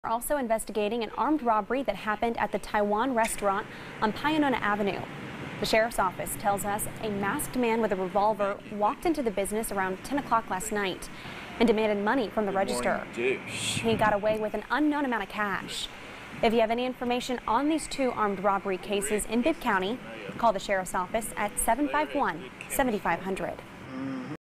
Macon restaurant robbed at gunpoint
Suspect escapes with cash from Tai-wan restaurant on Pio Nono Avenue